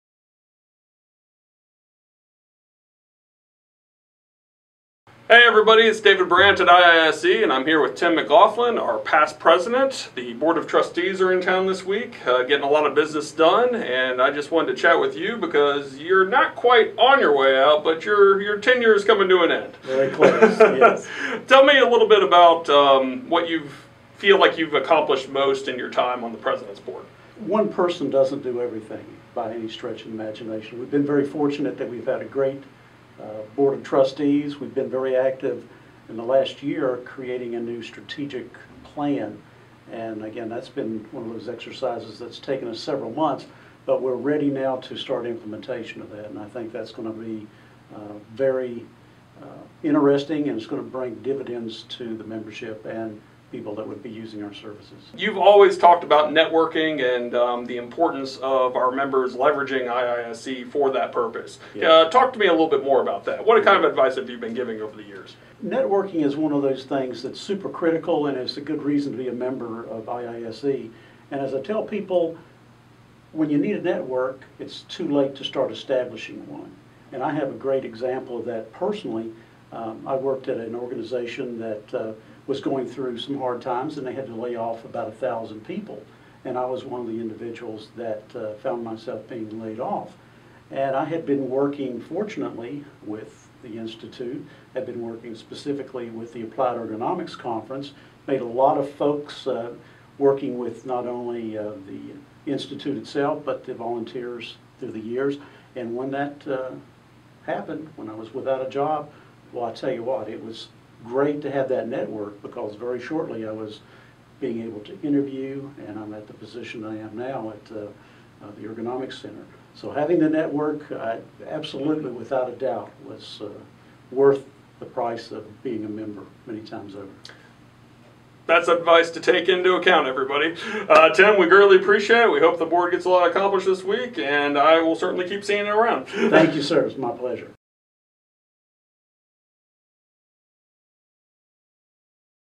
IISE Board of Trustees - Tim McGlothlin on networking with IISE
IISE Past President Tim McGlothlin explains how, after a period of downsizing, his IISE network helped him land on his feet at The Ergonomics Center of North Carolina.